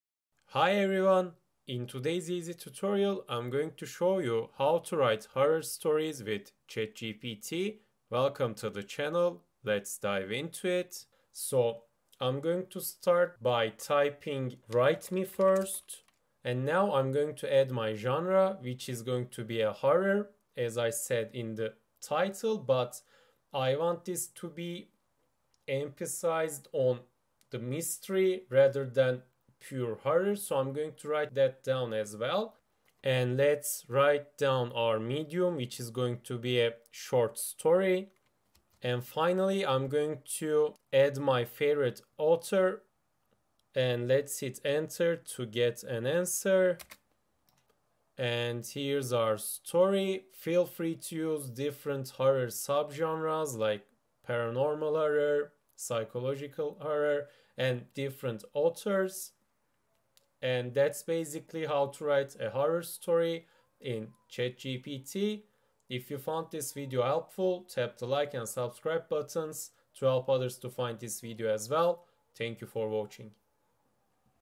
How To Use ChatGPT To Write Horror Stories Simple Tutorial
In this video I will show you how to use ChatGPT to write simple stories.